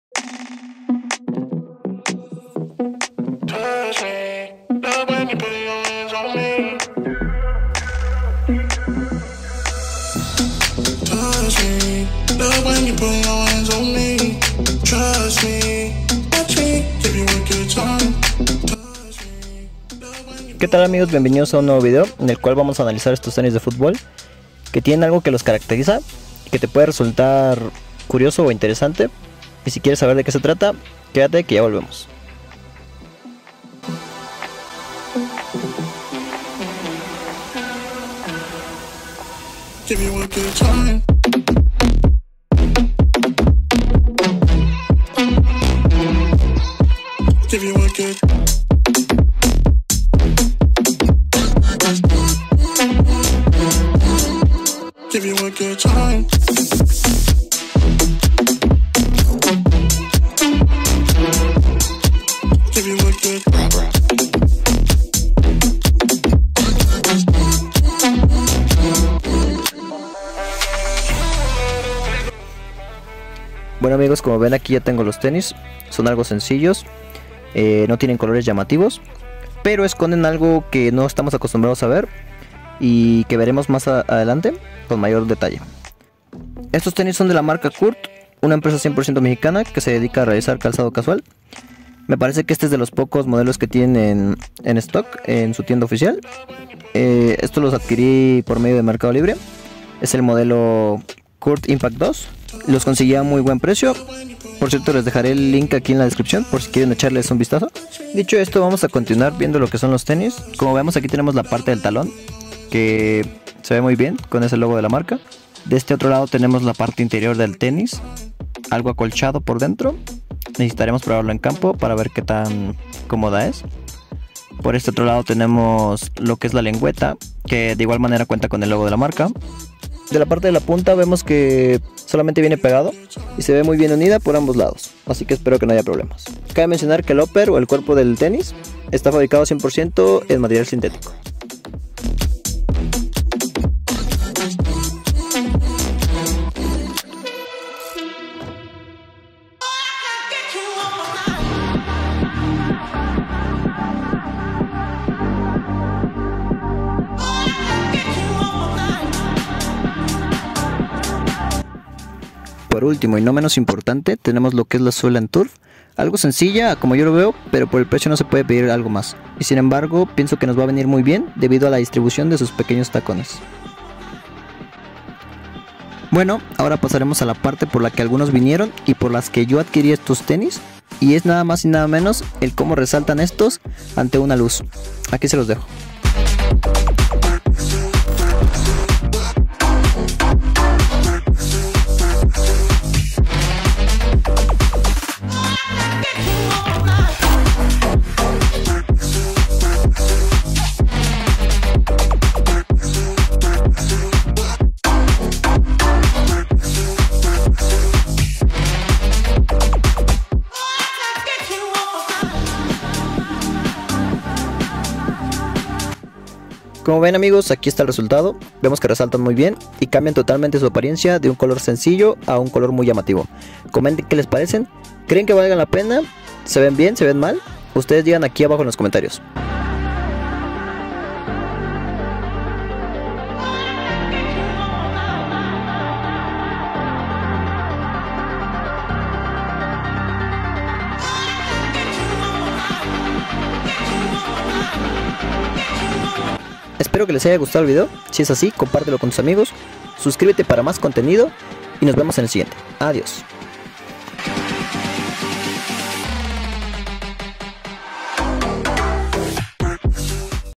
COURT IMPACT 2-UNBOXING Y REVIEW EN ESPAÑOL TENIS REFLEJENTES DE FUTBOL -TORNASOL//JOZU GOL MX
Court impact 2- Tenis tornasol